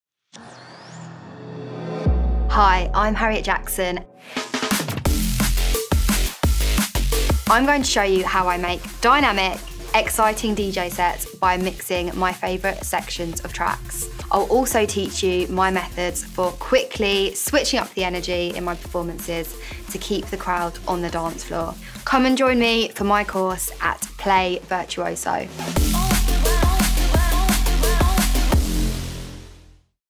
TRAILER: Harriet Jaxxon – Build high energy DJ sets
Master the art of modern music with expert guidance from the world’s best artists.